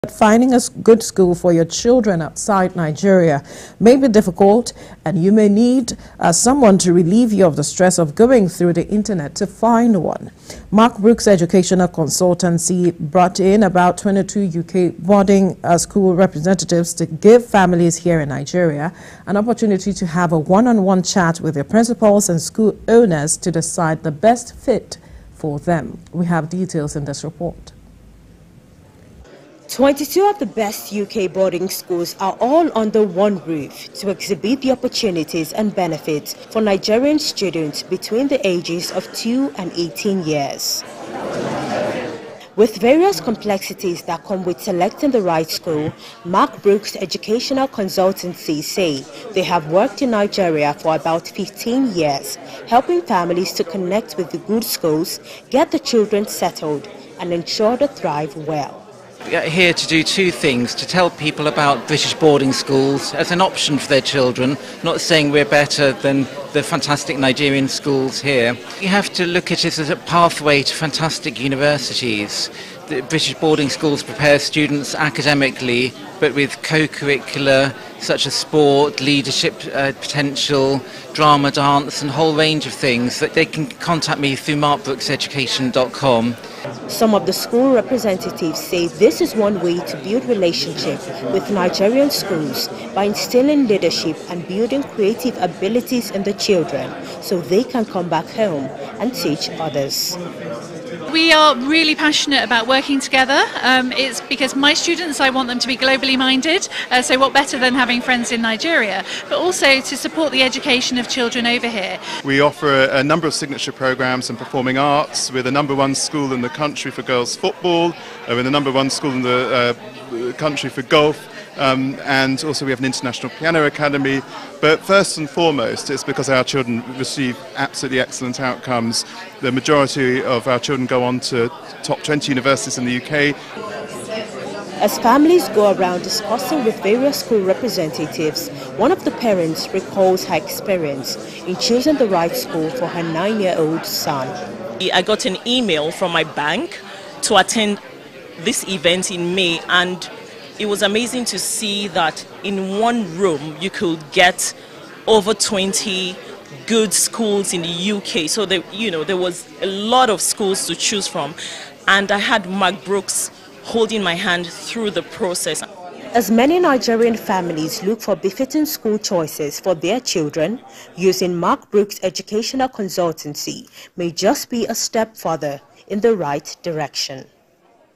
Mark Brooks Education Brings 22 Best UK Schools To Connect With Nigerian Families
Mark Brooks Educational Consultancy has brought in about 22 UK Boarding school representatives to give families here in Nigeria an opportunity to have a one-on-one chat with their principals and school owners to decide the best fit for them.